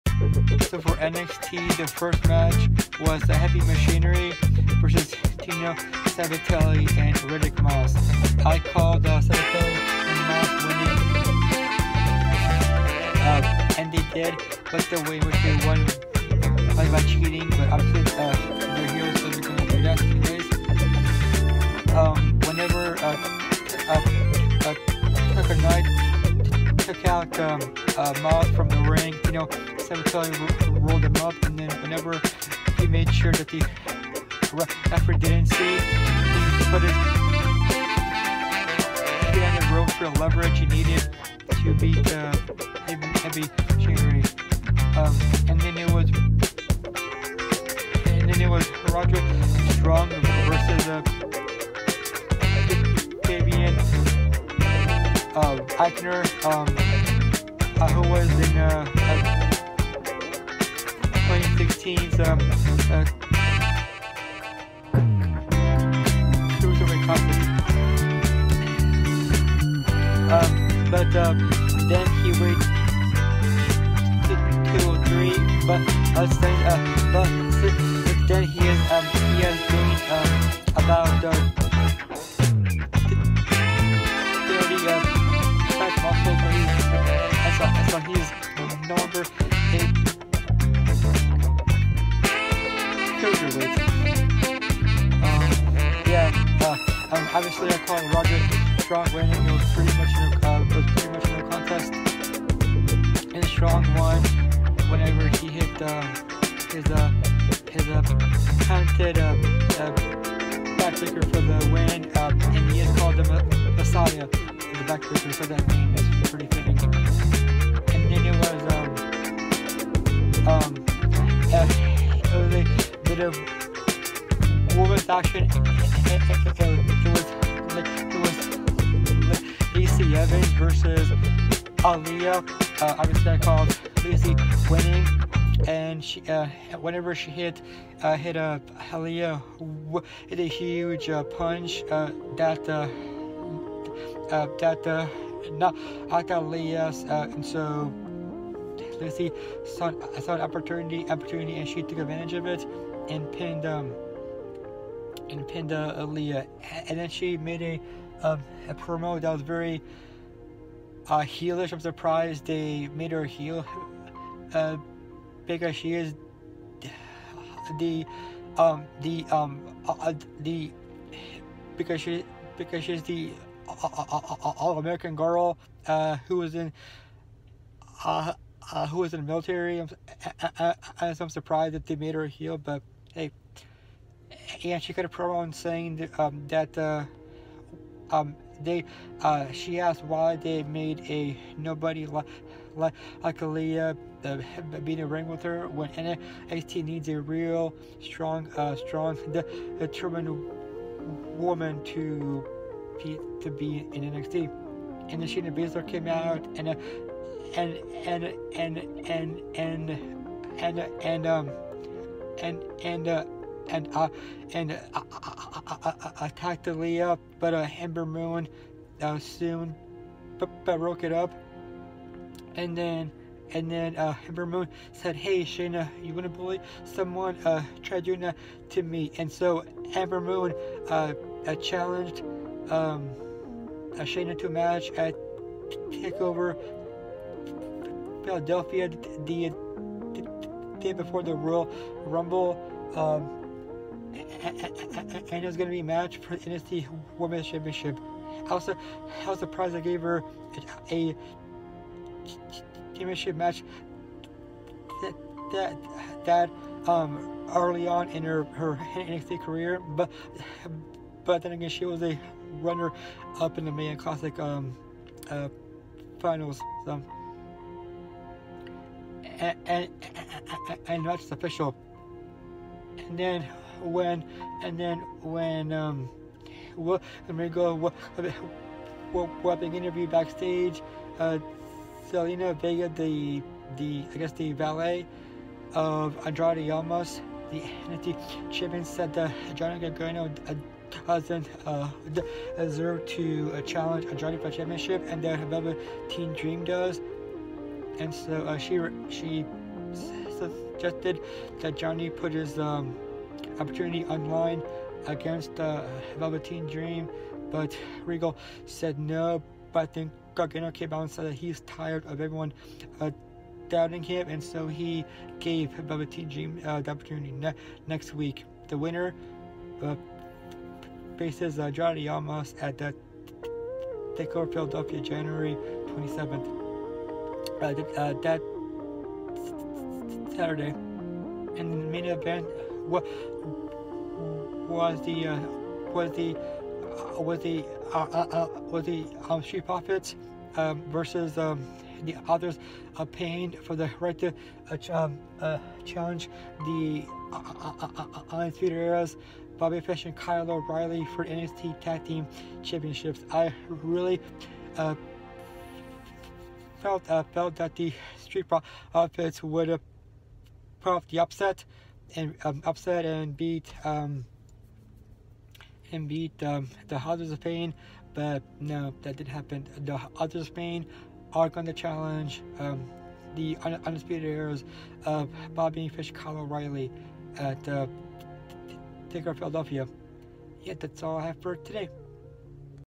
NXT Review 1/18/2018
I review this week's edition of NXT